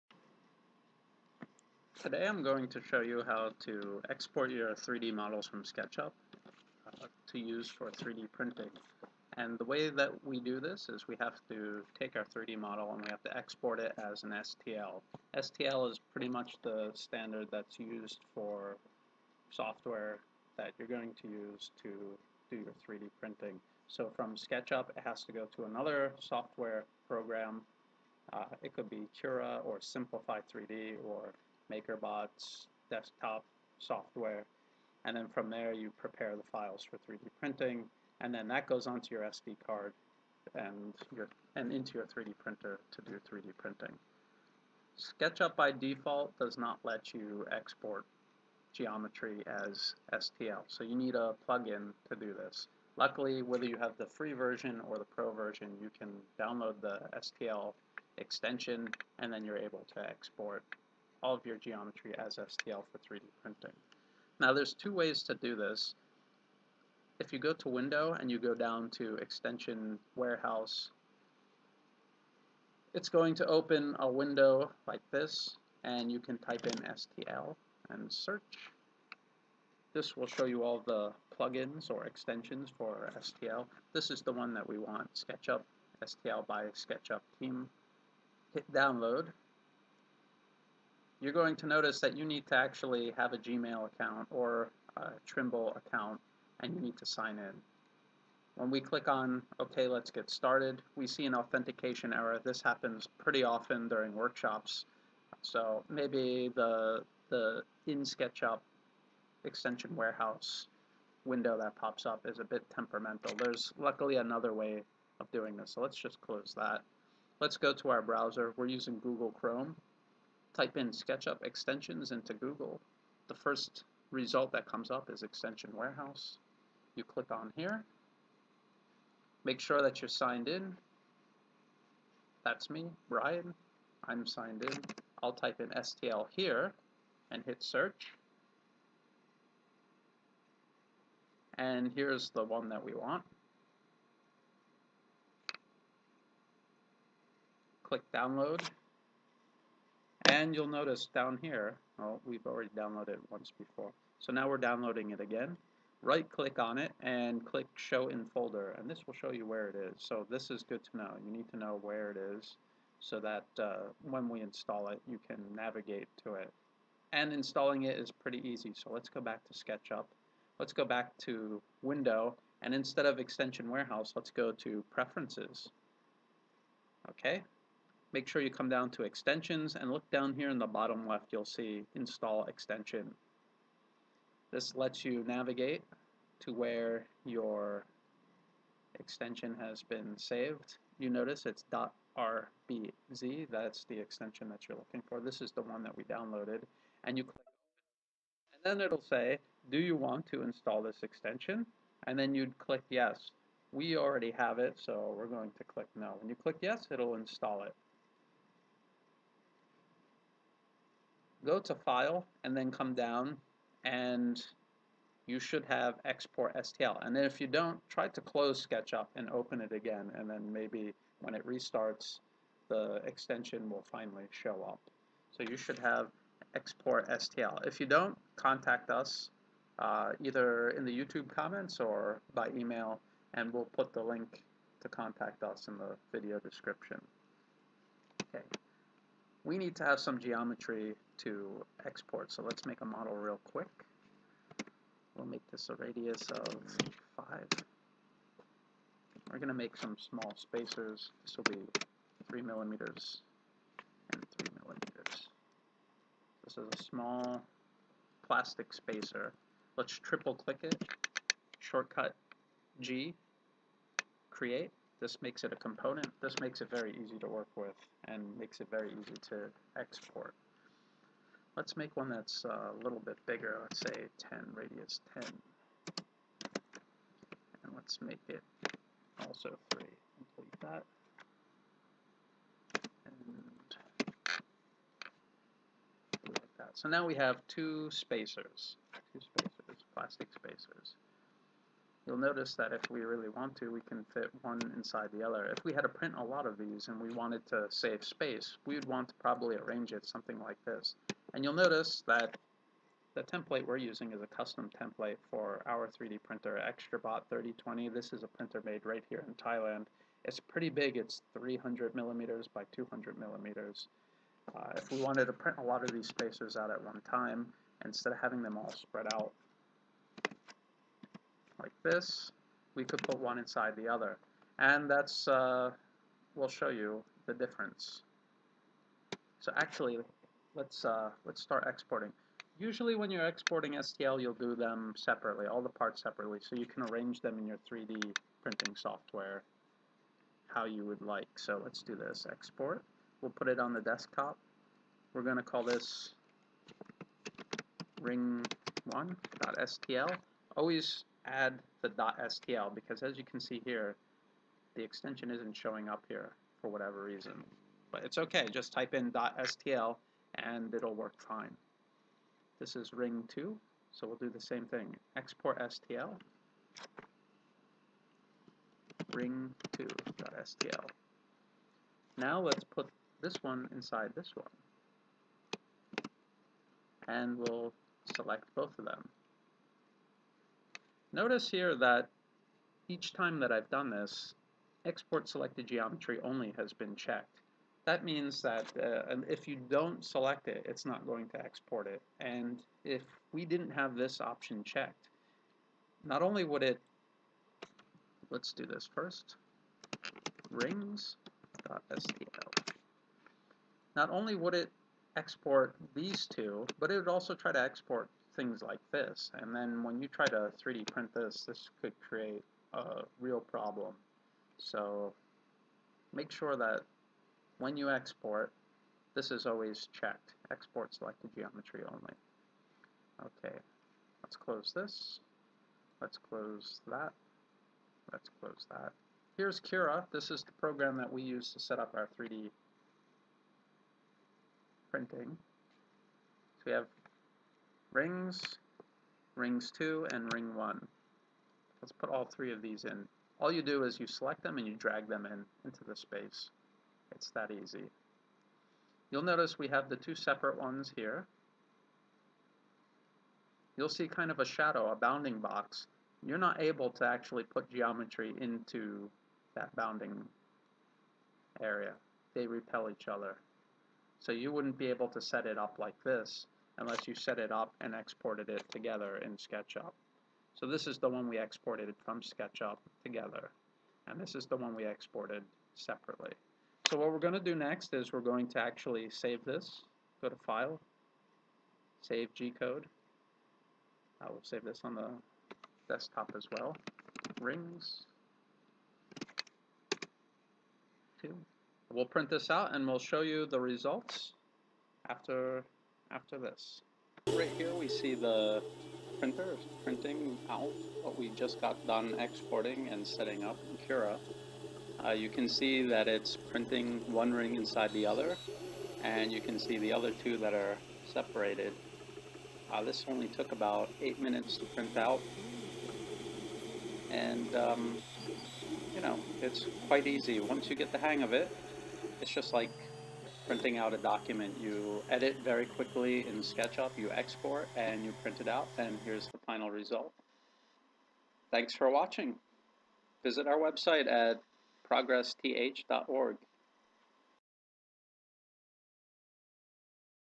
SketchUp + 3D Printing | Exporting STL Files
In this video, we explain how to install an STL file export plug-in for SketchUp and how to export your models for 3D printing.

We show you the entire process from making a simple model, to exporting it to Cura, to finally printing it out.